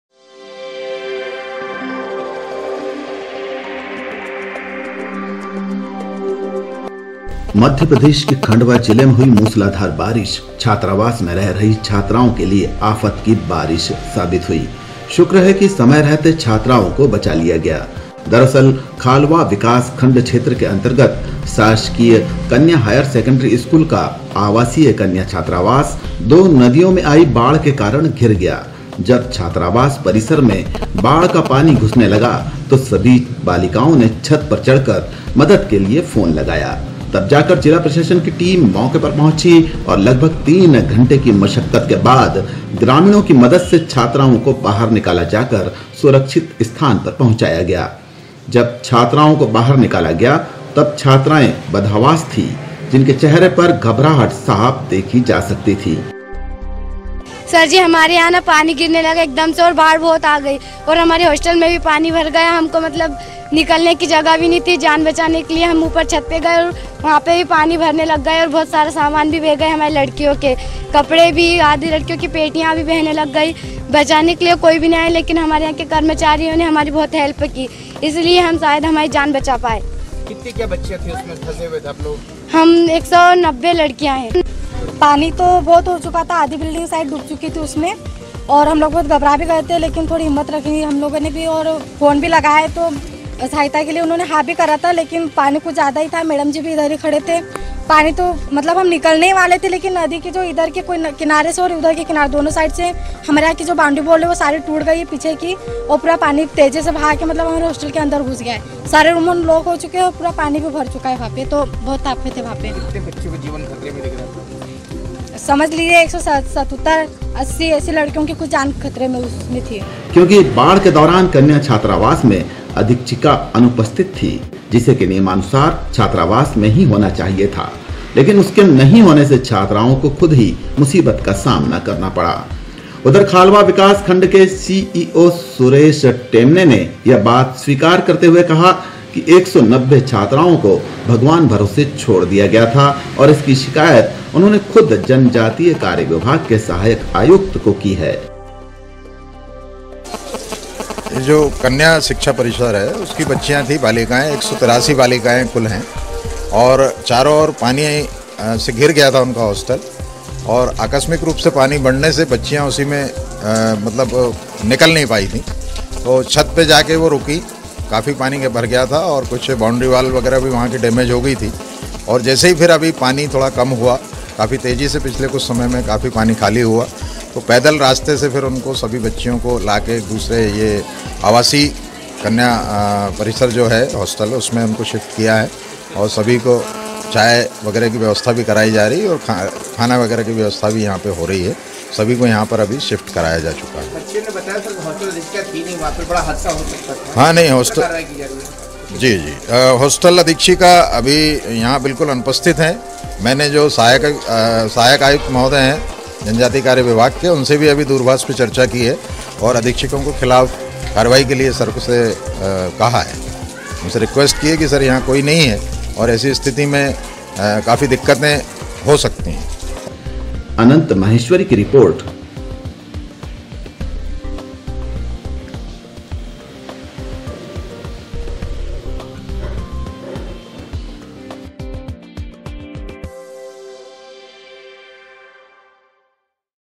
खंडवा-बाढ़ में फंसी एक सौ नब्बे छात्राओं को सुरक्षित निकाला MP NEWS NETWORK KHANDWA NEWS
मध्य प्रदेश के खंडवा जिले में हुई मुसलाधार बारिश,छात्रावास में रह रही छात्राओं के लिएआफत की बारिश साबित हुई. शुक्र है की समय रहते छात्राओं को बचा लिया गया .दरअसल खालवा विकासखंड क्षेत्र के अंतर्गत शासकीय कन्या हायर सेकेंडरी स्कूल का आवासीय कन्या छात्रावास दो नदियों में आई बाढ़ के कारण घिर गया . जब छात्रावास परिसर में बाढ़ का पानी घुसने लगा . तो सभी बालिकाओं ने छत पर चढ़कर  मदद के लिए फोन लगाया। तब जाकर जिला प्रशासन की टीम मौके पर पहुंची और लगभग तीन घंटे की मशक्कत के बाद  ग्रामीणों की मदद से छात्राओं को बाहर निकाला जाकर सुरक्षित स्थान पर पहुंचाया गया। जब छात्राओं को बाहर निकाला गया तब  छात्राएं बदहवास थी. उनके चेहरे पर घबराहट साफ़ देखी जा सकती थी . क्योंकि बाढ़ के दौरान कन्या छात्रावास में अधीक्षिका अनुपस्तिथ थी. जिसे की नियमानुसार छात्रावास में ही होना चाहिए था . लेकिन उनके नहीं होने से छात्राओं को खुद ही मुसीबत का सामना करना पड़ा ।
उधर खालवा विकासखंड के सीईओ सुरेश चंद टेमने  ने यह बात स्वीकार करते हुए कहा  कि एक सौ नब्बे  छात्राओं को भगवान भरोसे छोड़ दिया गया था  और इसकी शिकायत उन्होंने खुद जनजातीय कार्य विभाग के सहायक आयुक्त को की है।